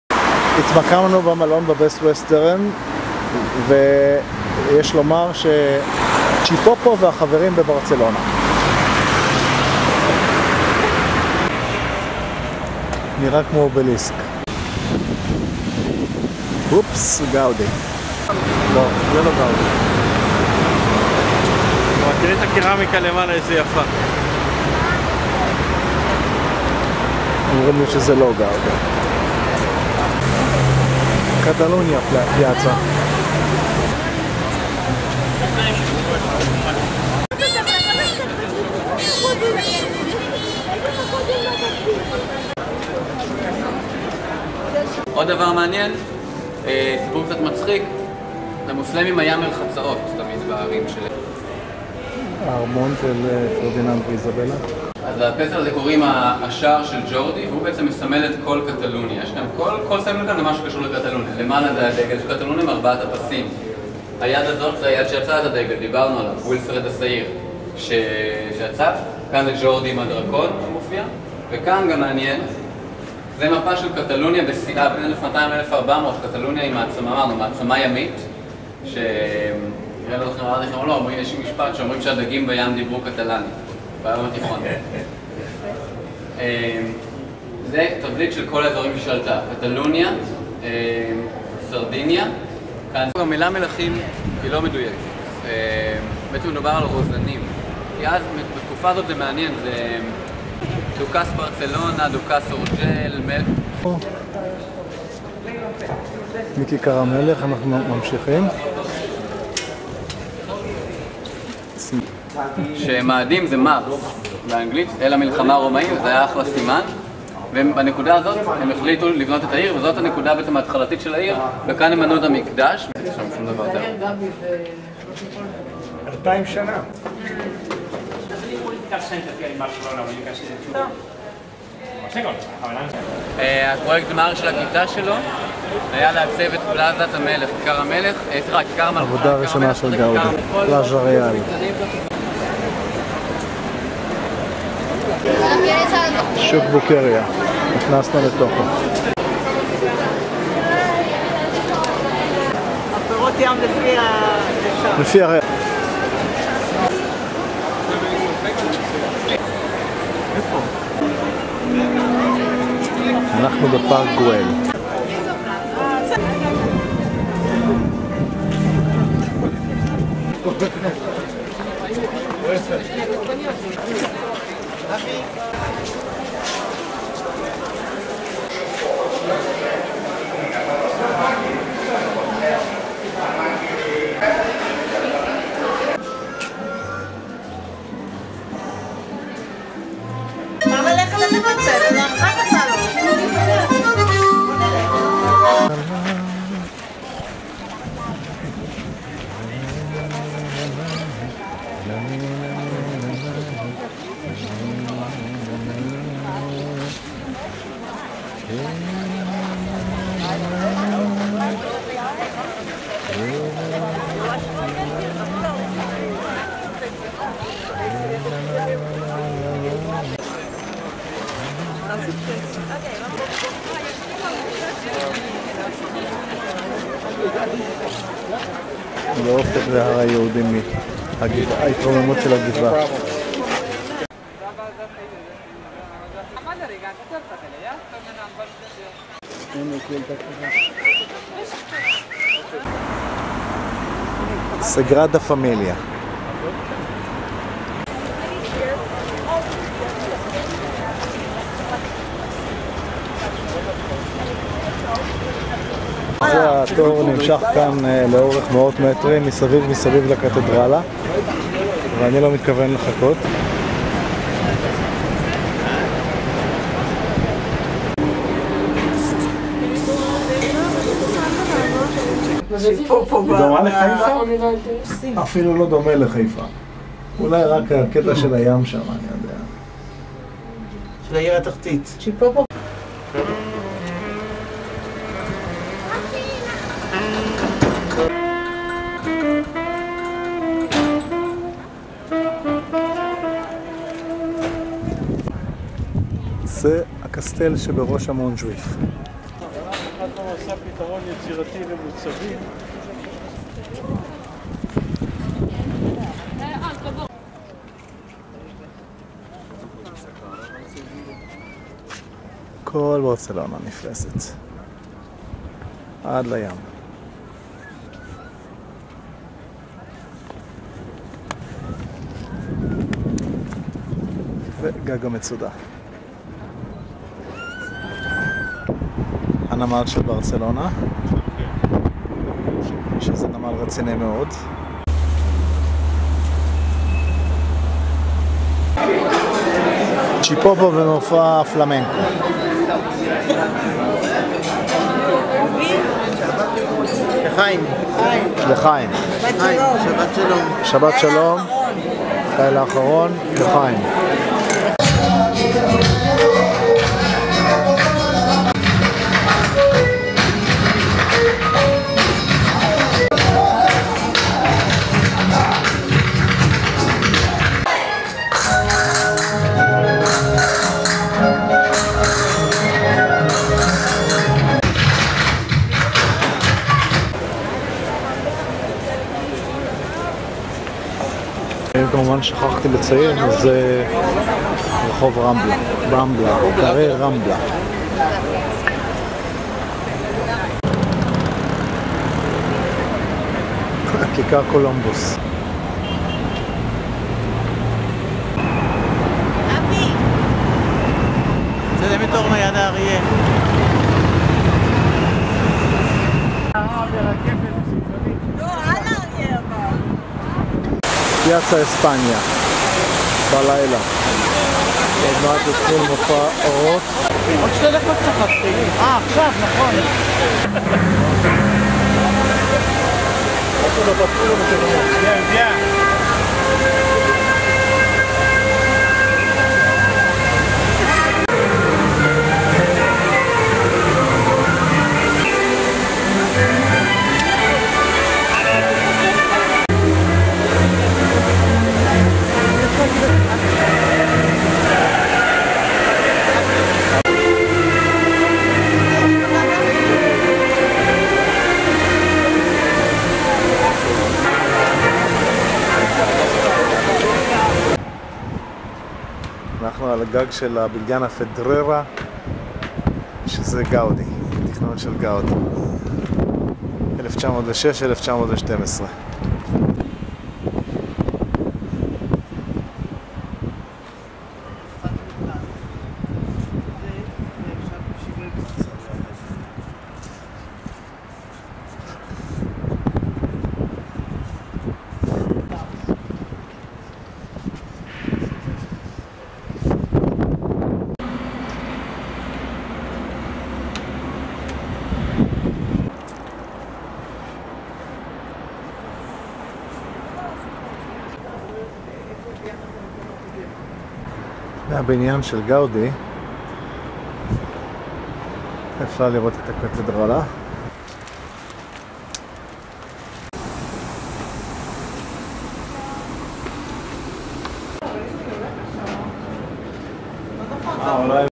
Barcelona - Part 2
A group travel to the Catalunians Pirenees Mountains. organized by Med Raft guided by offer. Set out from Barcelona on August 24, 2012. This being the second part : returning to Barcelona. Continue the last evening with the group, plus 2 more nights stay in the city to the Tals (us) and the Tzichinsky families.